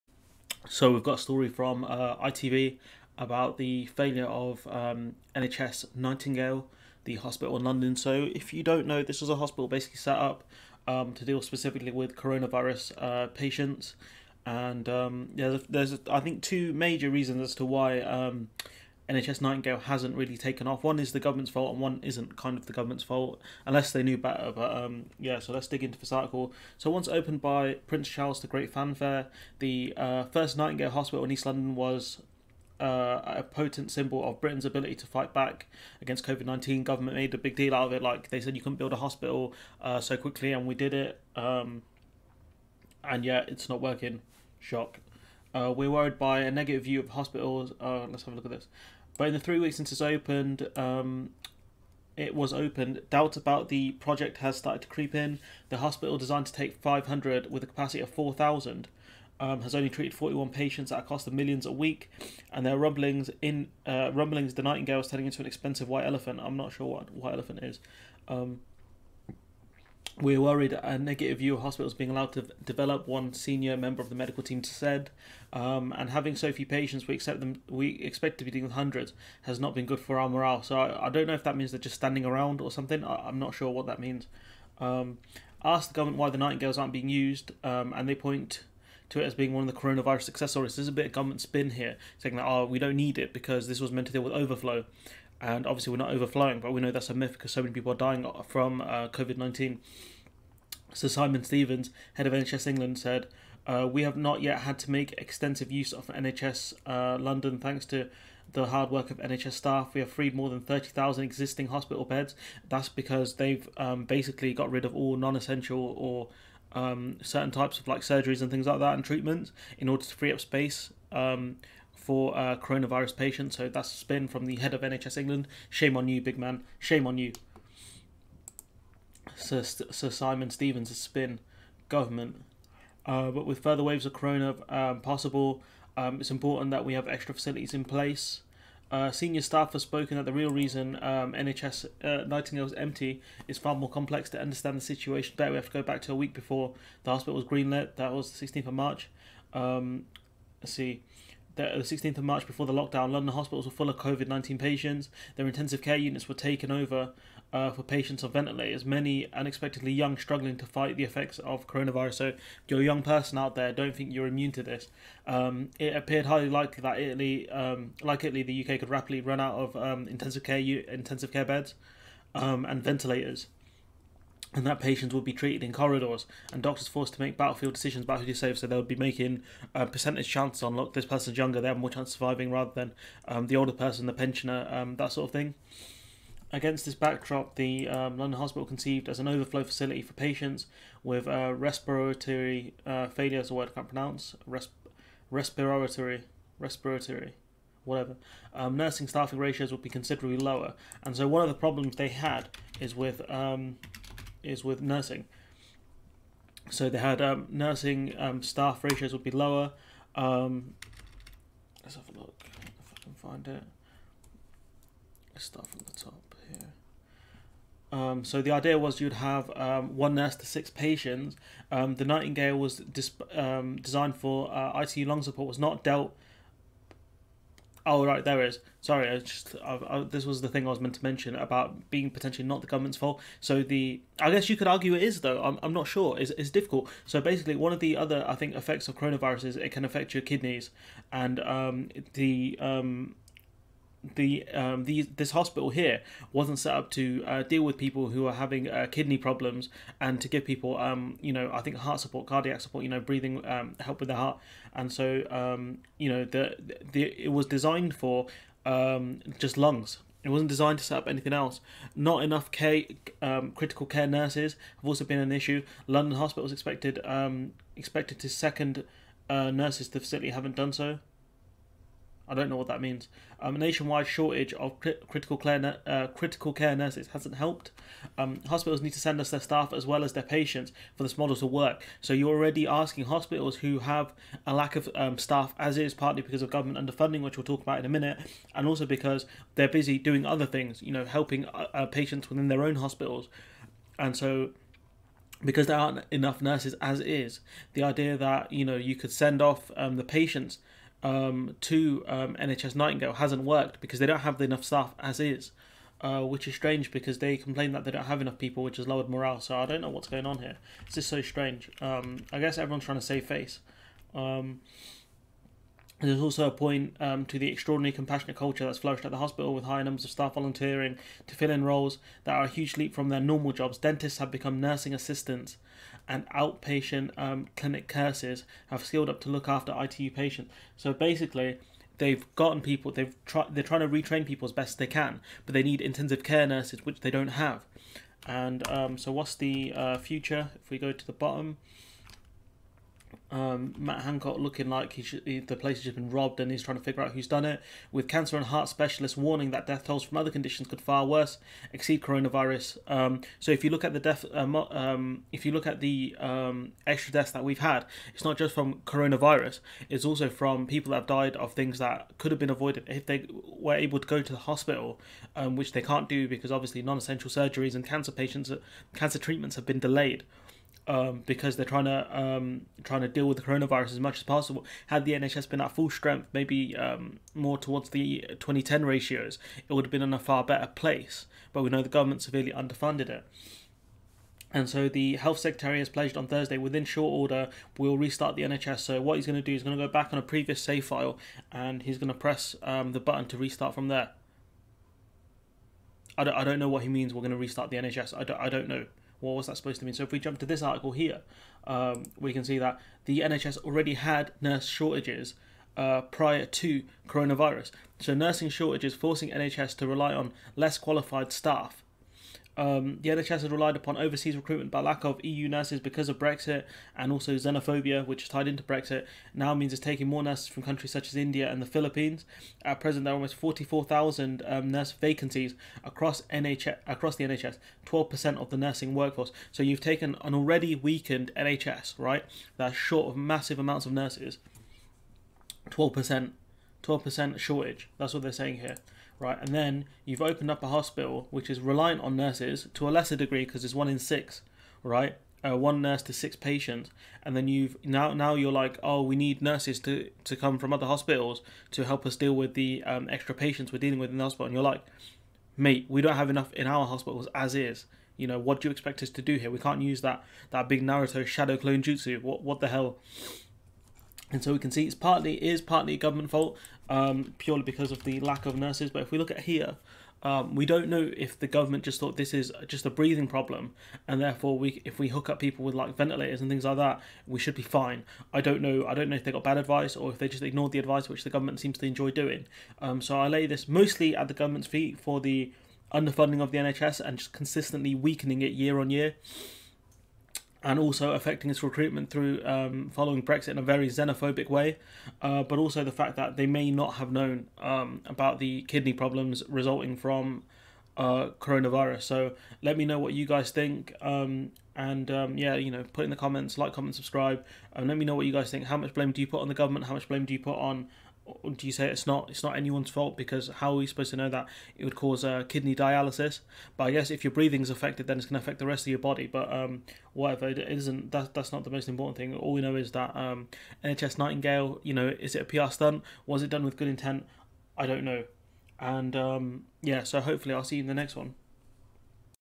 NHS Nightingale not working properly. Partially the government's fault partly not.
The NHS Nightingale hospital is not working to anywhere near full capacity. Government trying to take a victory lap but we know that coronavirus death are high and because of nurse shortage caused by the Tory government. The video was a pain to render but had to be done.
Articles used:
Nursing shortages forcing NHS to rely on less qualified staff – report